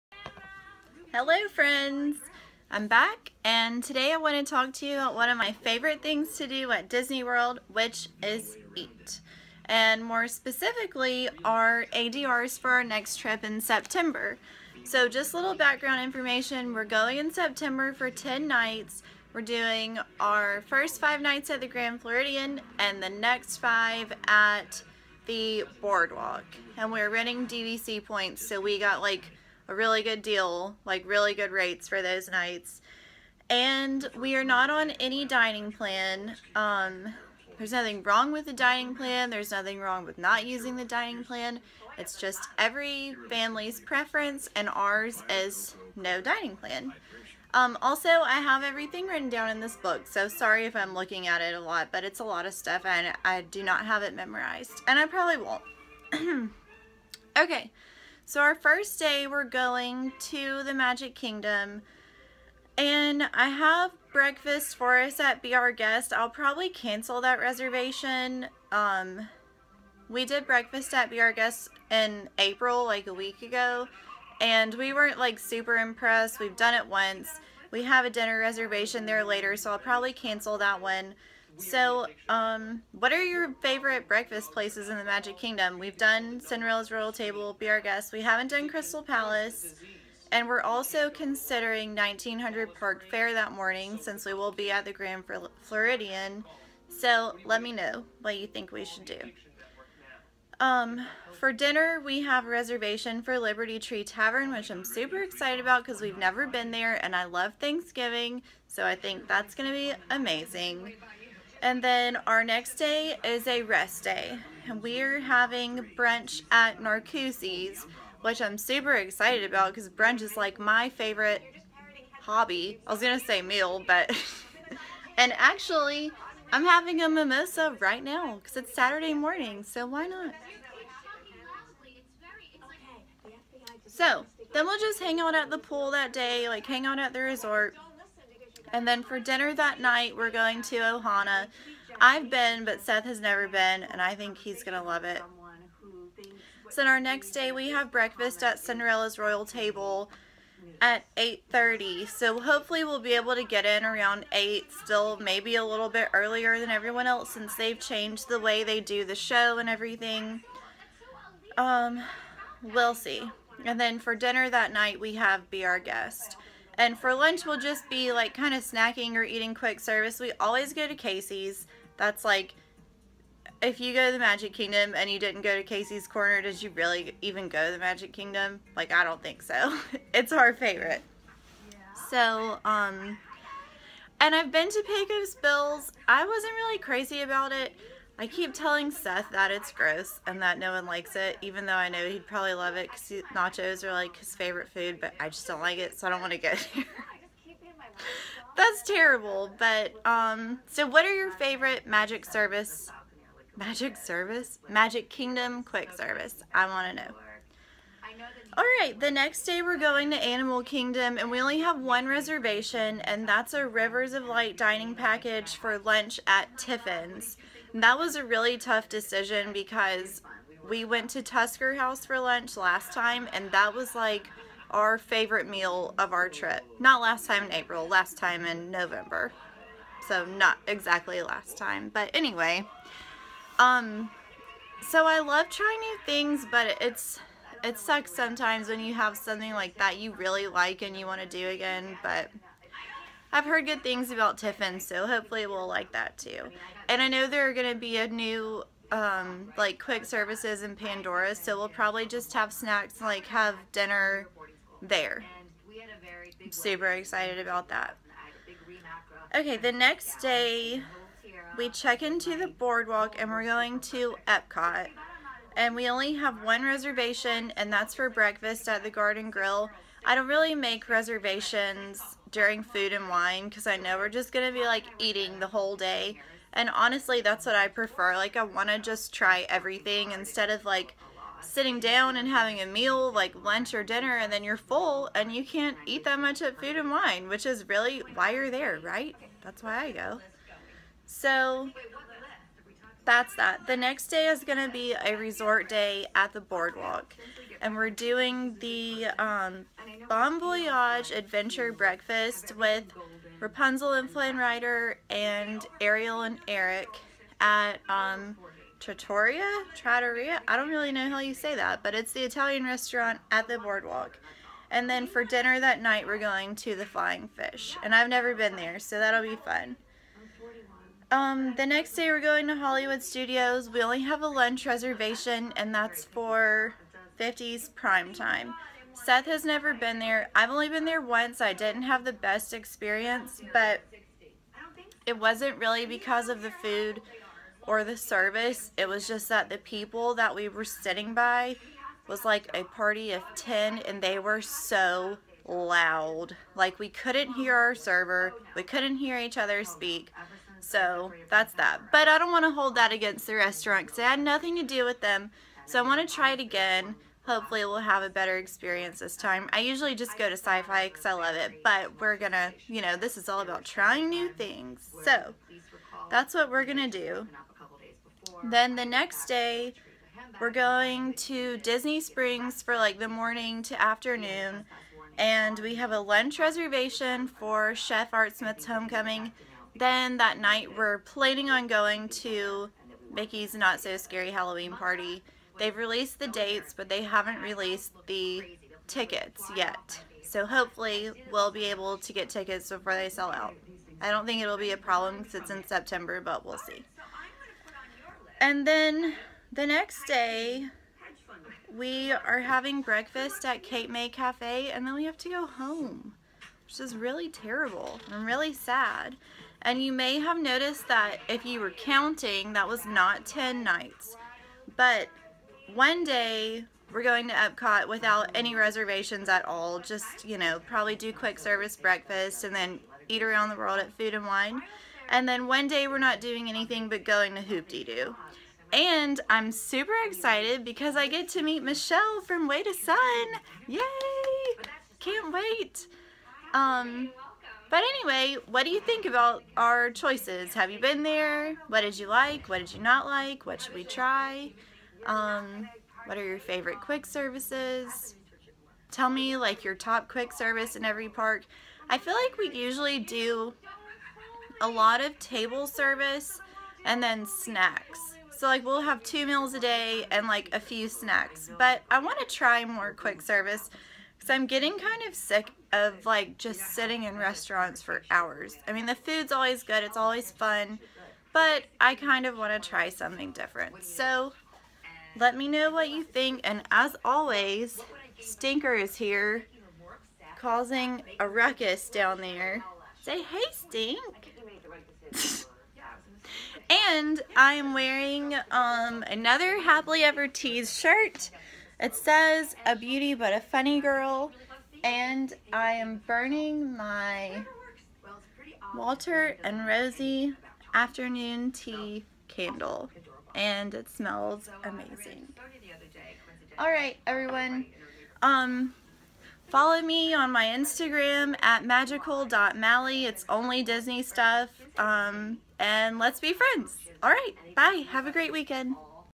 Walt Disney World ADR's!
Hello Disney friends! In this video I share with you our ADR's for our upcoming trip in September! Please let me know if you've been to these reservations and what you thought! Also please comment your favorite quick service at each park!

Also, because I know someone will ask, the blanket behind me is just a little throw blanket that I got on the shop parks app! 😊